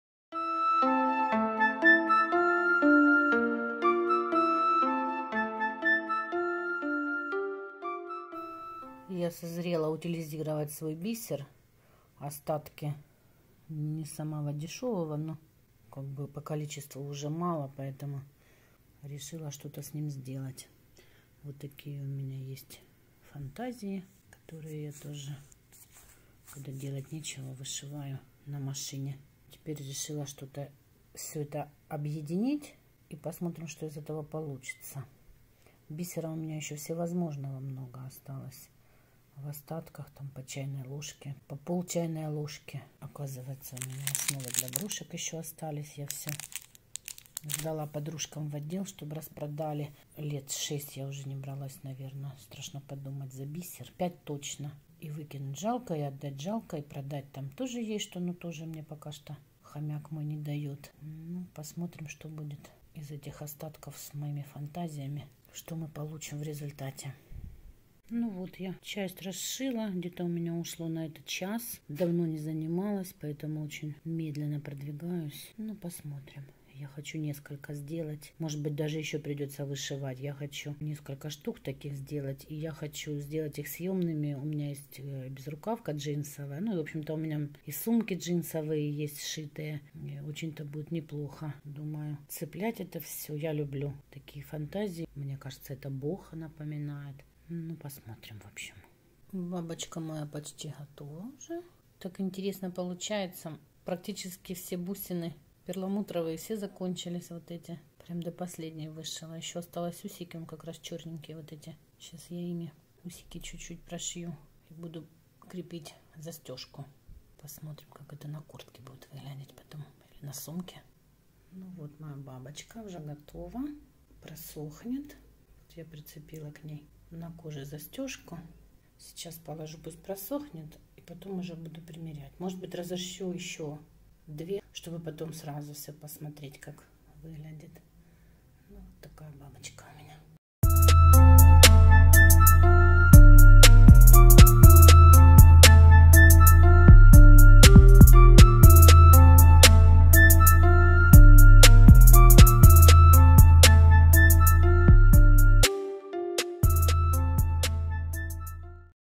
Утилизация остатков бисера и джинсовых лоскутов ♥ Брошь Бабочка своими руками ♥ MilaBalitskaya
Утилизирую остатки бисера и джинс. Вышиваю бабочек, оформляю броши ♥